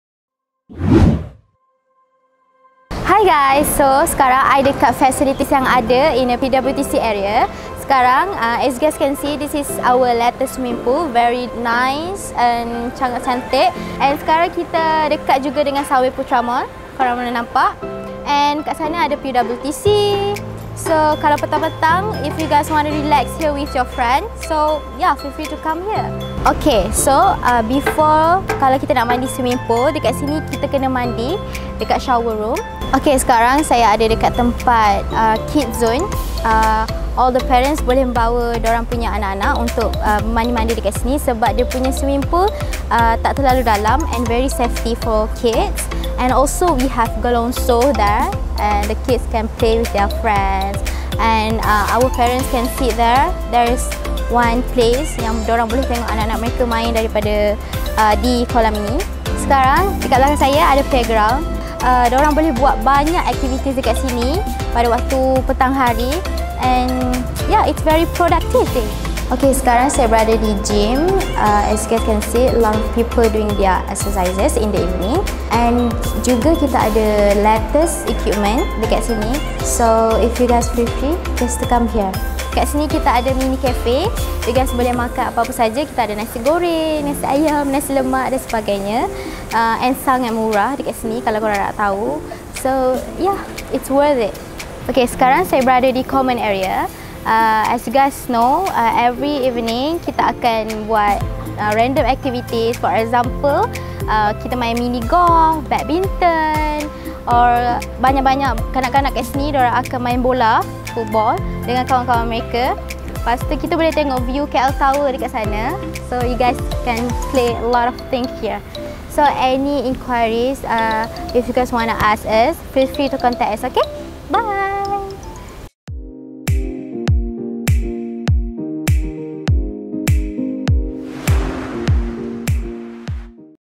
Utopia @ PWTC - Facilities
In this episode we would like to share our facilities to all of you amazing people who is interested on visiting Utopia PWTC. In this place we have many facilities that fits all of your need while staying here whether if you need to exercise and keep fit your body, we provide gym facility. Need a place to spend time with your friends and love ones? We have a common area where you have activities such as badminton, football and etcetera while having an amazing KL view.

Interested about this place? Check out Utopia to find more rooms to rent in Kuala Lumpur.